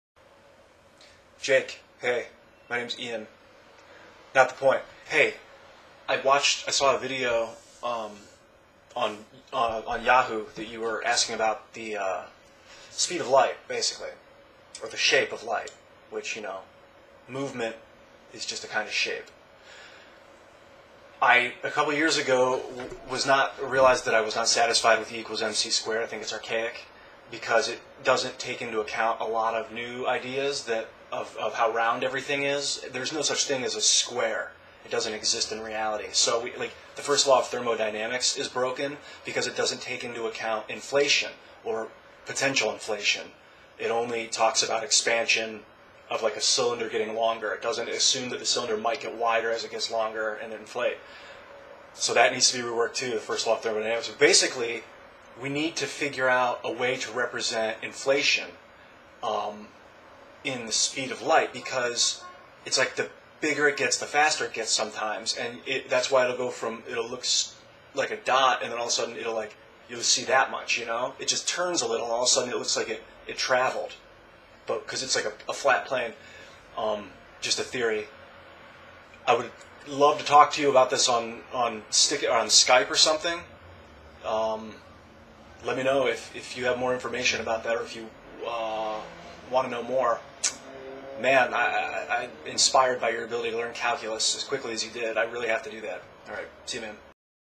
To Jake: Re: The speed/shapechange of light
This video is barely a complete thought.  I think c^2 is representative of light emanating outward from a center point, which is not how light behaves.  It is very directional and looks different from different angles.

I'm not sure if incorporating a shape change unit like delta would work.  I have some stuff written from a few years back on myspace.  It's a bit non-sciency but can probably be translated into an equation with the right approach.